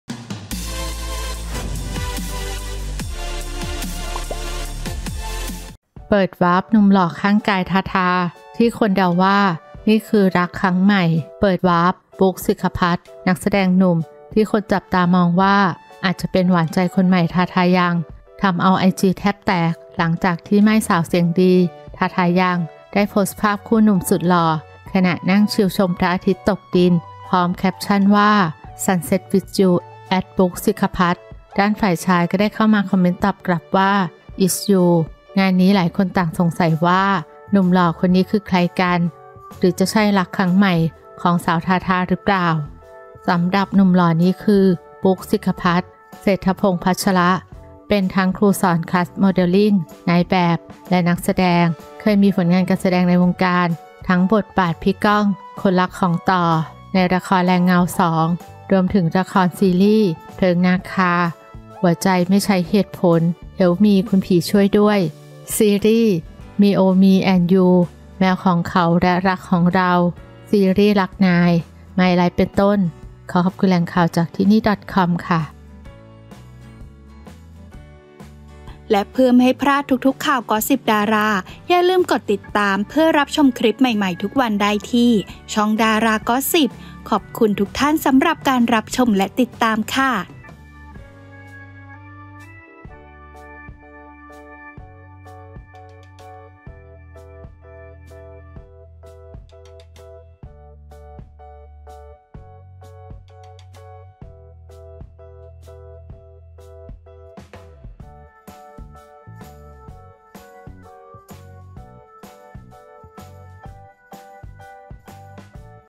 เปิดวาร์ปหนุ่มหล่อข้างกาย ทาทา ที่คนเดาว่านี่คือรักครั้งใหม่
เปิดวาร์ป บุ๊ค สิคพัชศ์ นักแสดงหนุ่มหนุ่มที่คนจับตามองว่าอาจจะเป็นหวานใจคนใหม่ทาทา ยัง ?
ทำเอาไอจีแทบแตก!! หลังจากที่หม้ายสาวเสียงดี ทาทา ยัง ได้โพสต์ภาพคู่หนุ่มสุดหล่อขณะนั่งชิลชมพระอาทิตย์ตกดินริมทะเล พร้อมแคปชั่นว่า ' Sunset with You! @booksiqaphat' ด้านฝ่ายชายก็ได้เข้ามาคอมเมนต์ตอบกลับว่า 'Is u'
งานนี้หลายคนต่างสงสัยว่าหนุ่มหล่อคนนี้คือใครกัน? หรือจะใช่รักครั้งใหม่ของสาวทาทาหรือเปล่า?
สำหรับหนุ่มหล่อคนนี้คือ บุ๊ค สิคพัชศ์ เศรษฐพงพัชร เป็นทั้งครูสอนคลาสโมเดลลิ่ง นายแบบ และนักแสดง เคยมีผลงานการแสดงในวงการมาแล้วไม่น้อย ทั้งบทบาท "พี่ก้อง" คนรักของ ต่อ (ป๊อบ พิชฌพัฒน์) ในละคร แรงเงา2
รวมถึงละคร และซีรีส์ เพลิงนาคา, หัวใจ...ไม่ใช้เหตุผล , Help me คุณผีช่วยด้วย, ซีรีส์ MEO Me & You แมวของเขาและรักของเรา , ซีรีส์รักนาย myride ฯลฯ